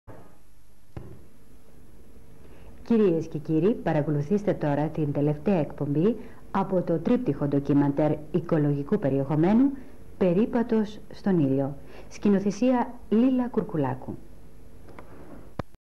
ΜΙΧΕΛΗ ΡΑΝΙΑ
Αναγγελία προγράμματος από την τηλεπαρουσιάστρια της ΕΤ-1
Ράνια Πέτσα-Μιχελή. Μεταδόθηκε το 1991.